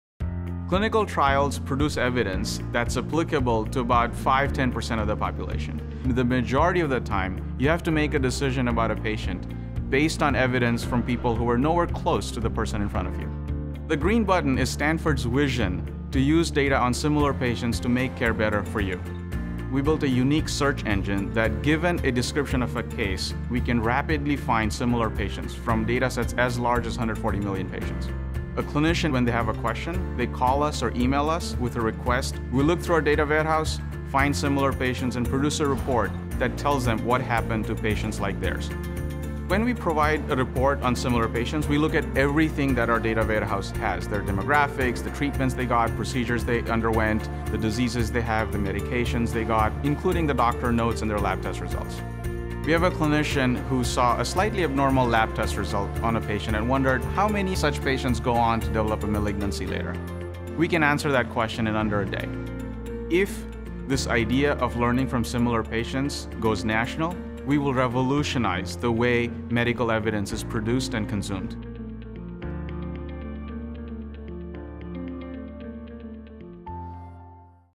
Green button, and the informatics consult service
The Informatics Consult is an IRB approved project to study the use of routinely collected data on millions of individuals for providing better care.